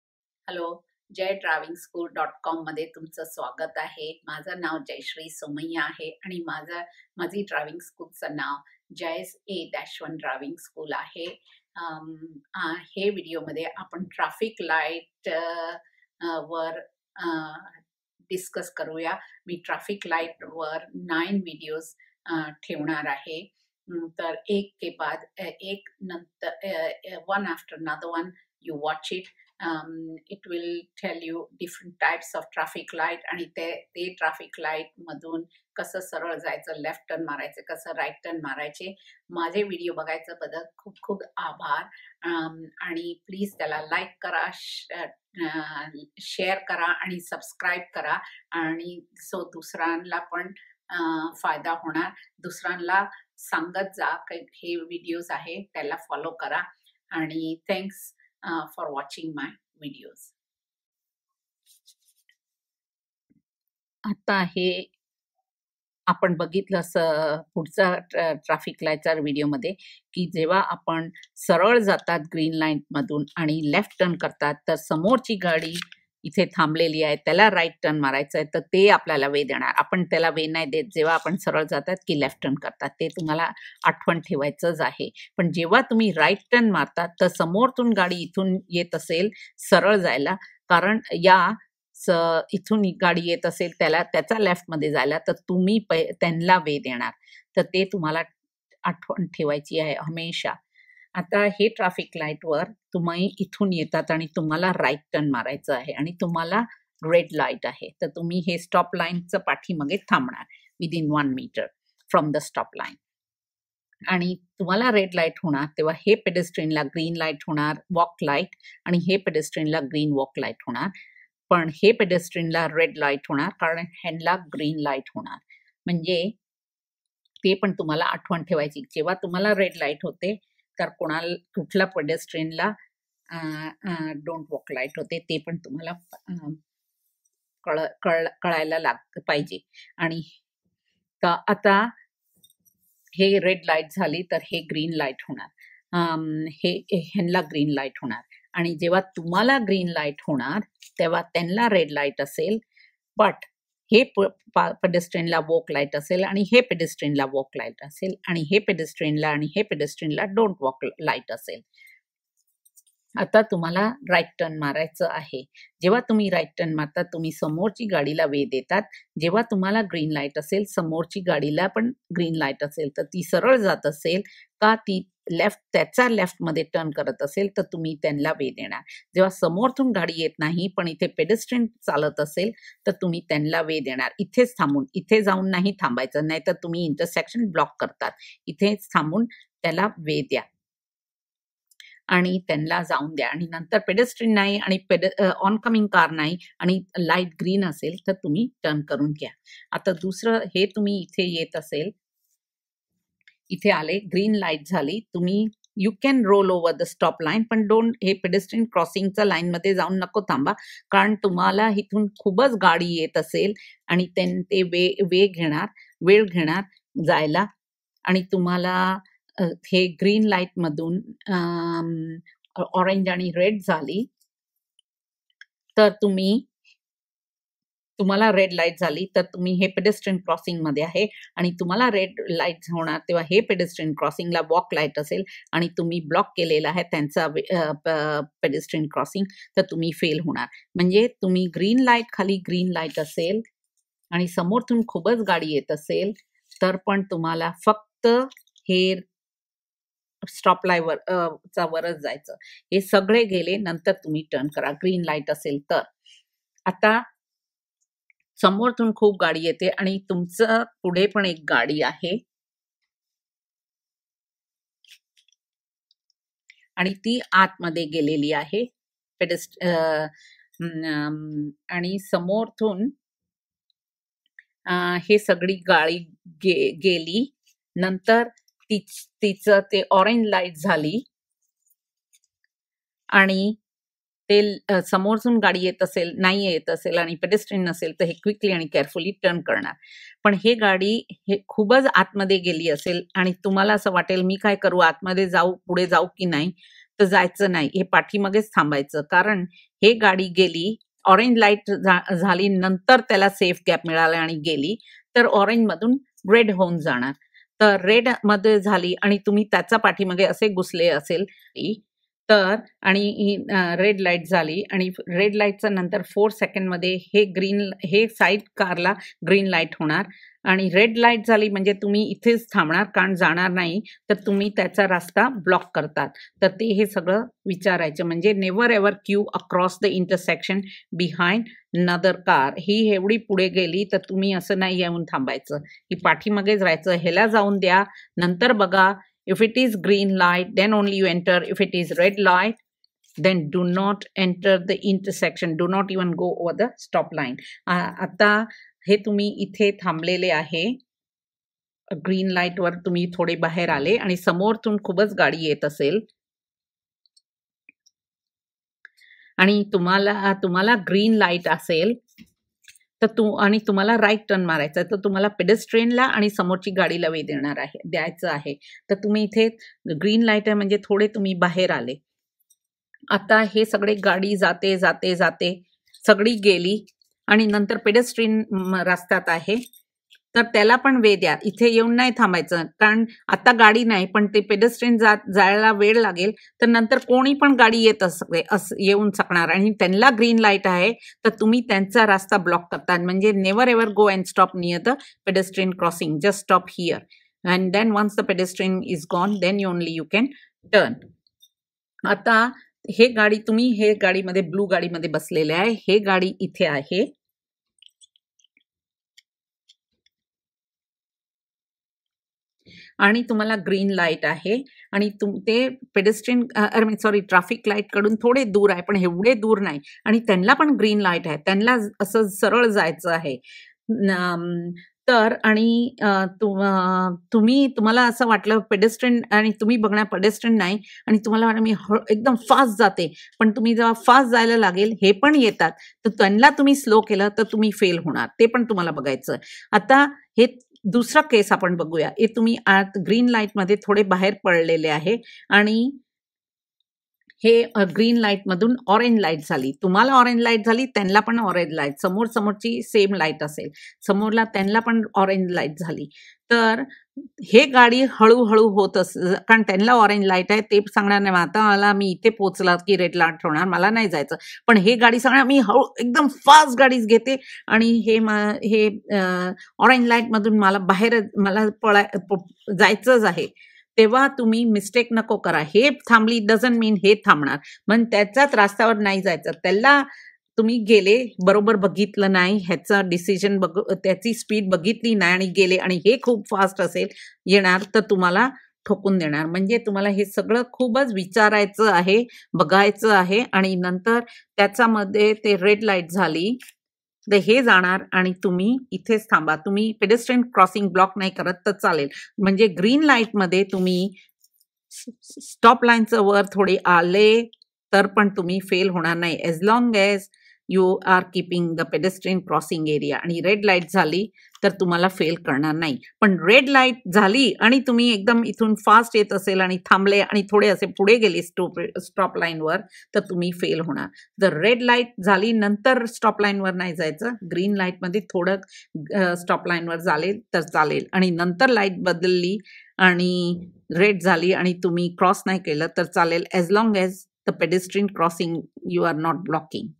Episode 20.2 Traffic Light (Theory) ( Marathi)
Diffrent types of traffic lights and how to go through them.

These driving rules are based in New South Wales, Australia.